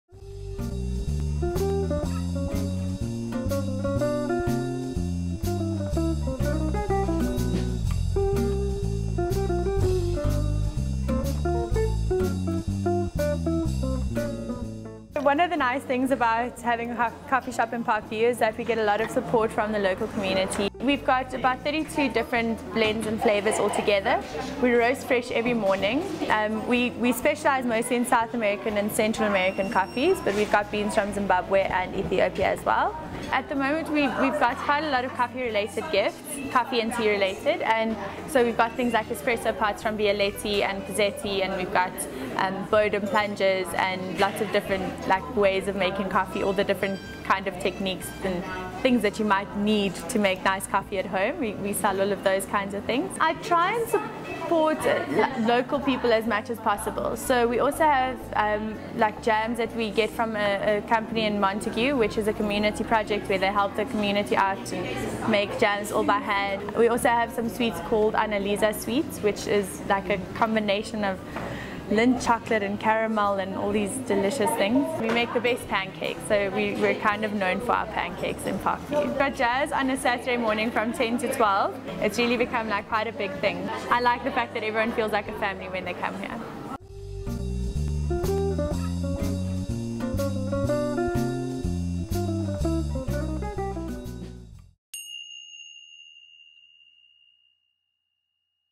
The Perfect Cup coffee shop in Parkview
Michelle van Blerk owns and runs The Perfect Cup coffee shop in Parkview. We are regular customers and can attest to the fact that Michelle does fantastic coffee!